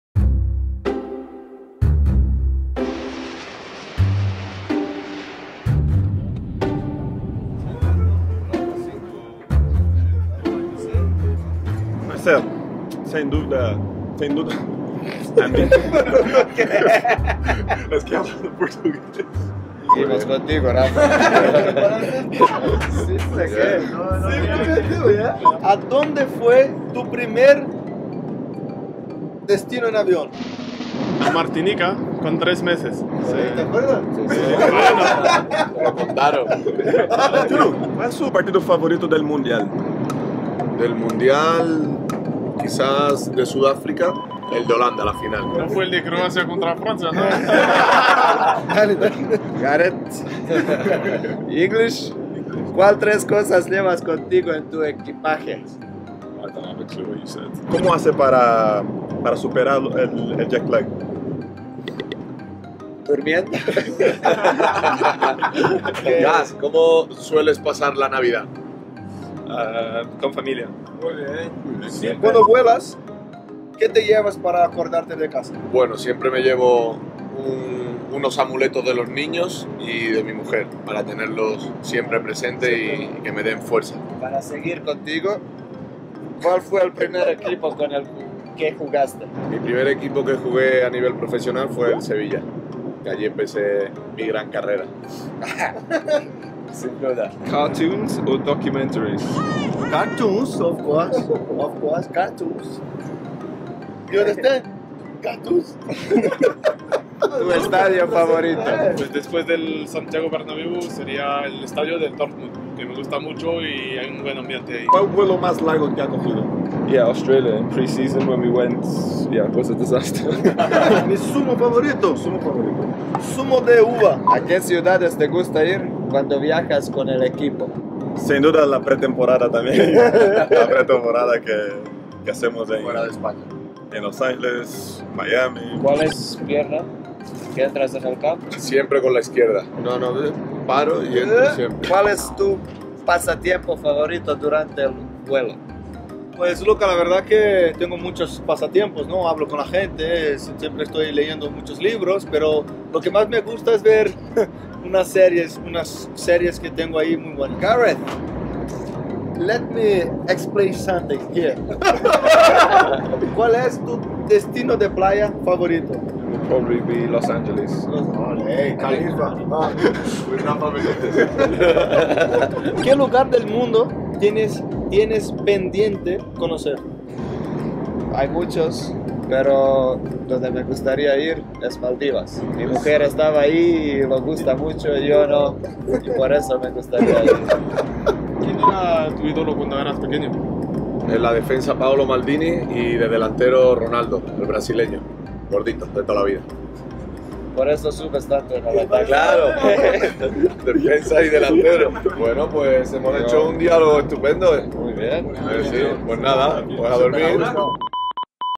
MARCELO, BALE, RAMOS and their teammates
FUNNY MOMENTS Emirates A380!
Find out how Sergio Ramos, Marcelo, Gareth Bale, Luka Modric, Raphael Varane and Casemiro pass the time while travelling on the Emirates A380 in this hilarious video!